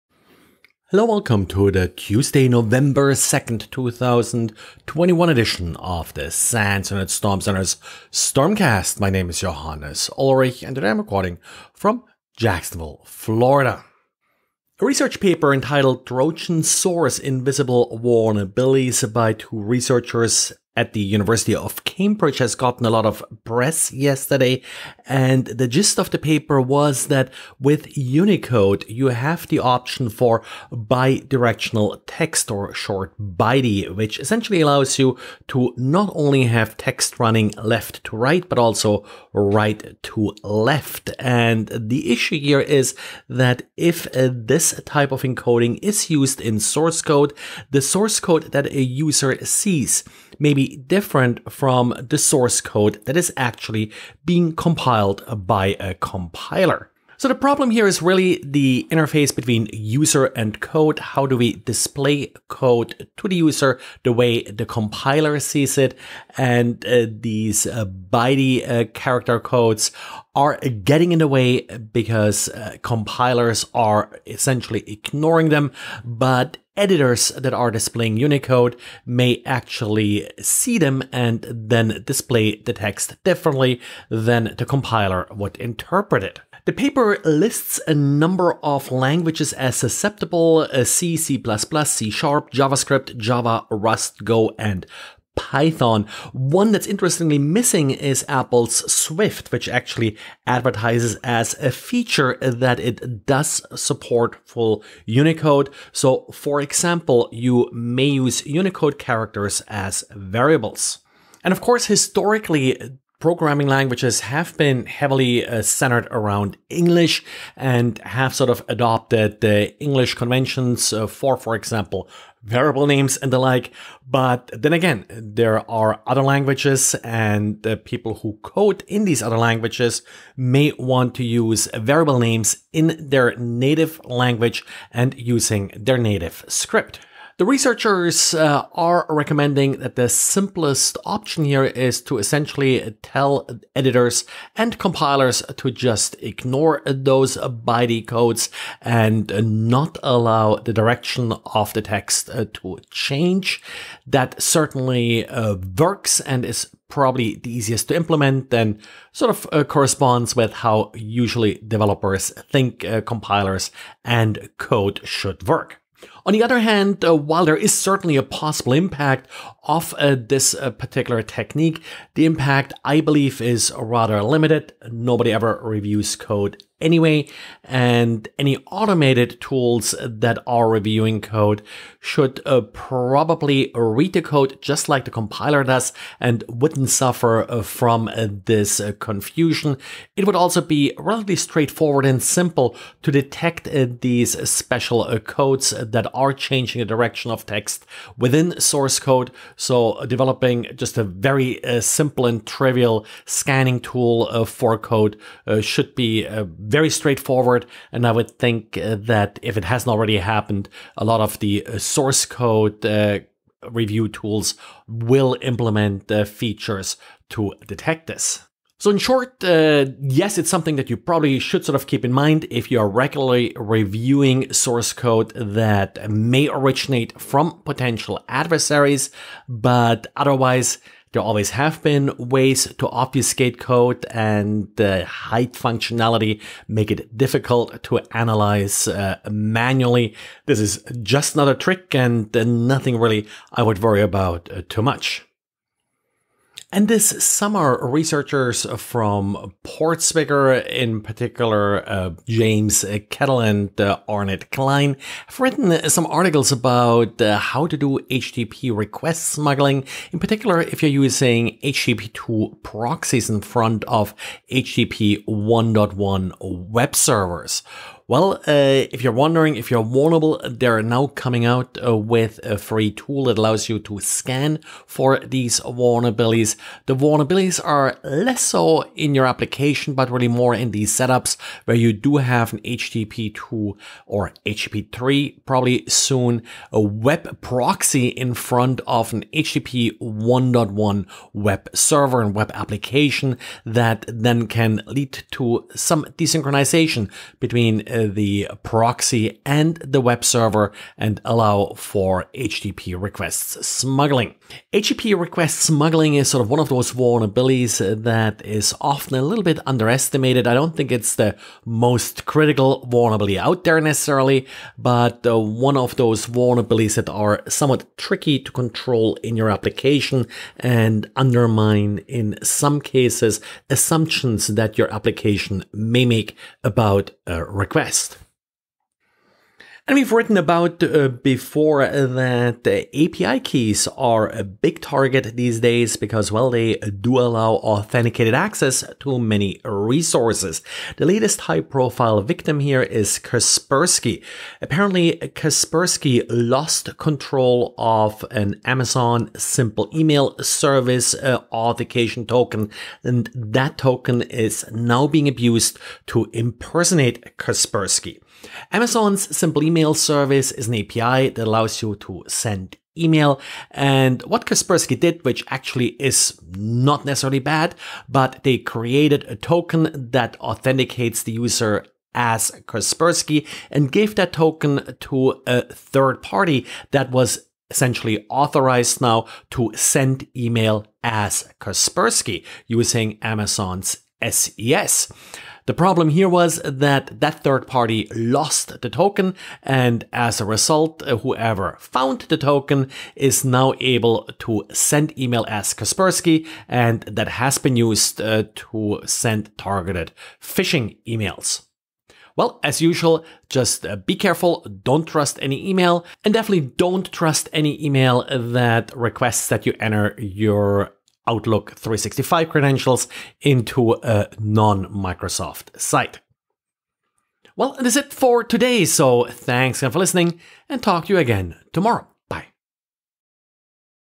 Network Security News Summary for Tuesday November 2nd, 2021
Hiding Source Code; Detecting Header Smuggling; Kaspersky AWS SES Token Lost